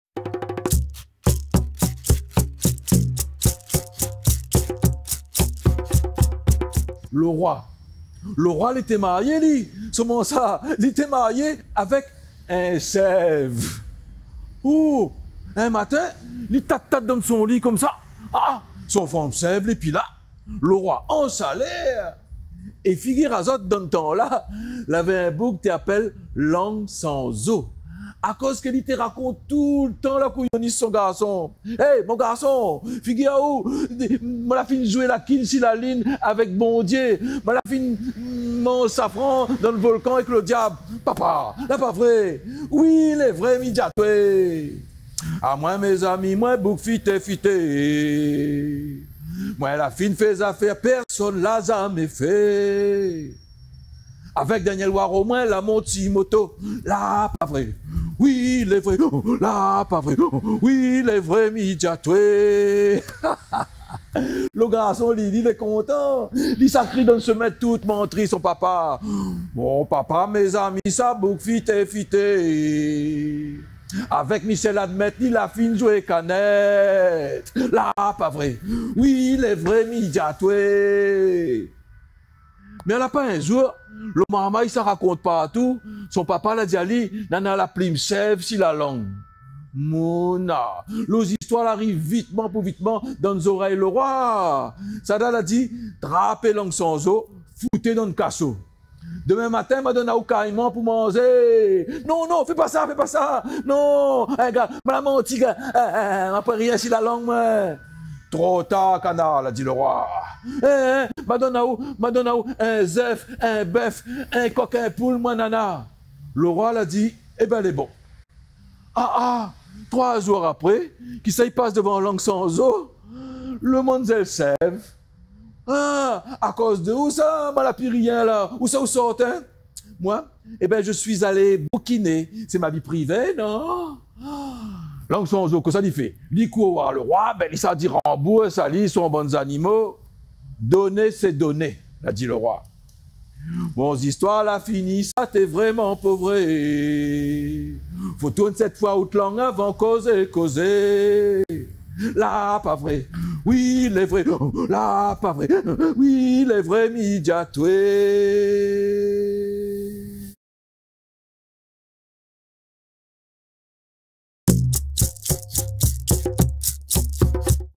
Pastille Contée | La lang i pandi domoun - Sully Andoche
[PASTILLE CONTÉE] 🎭 Rendez-vous tous les mercredis à 16h pour votre nouvelle pause culturelle ! Chaque semaine, un conteur succédera à un autre pour vous faire voyager au pays des contes. Ce mercredi, Sully Andoche vous raconte l’histoire de « La lang i pandi domoun ».

Alon fé viv nout patrimoine, partaz a li 😉 !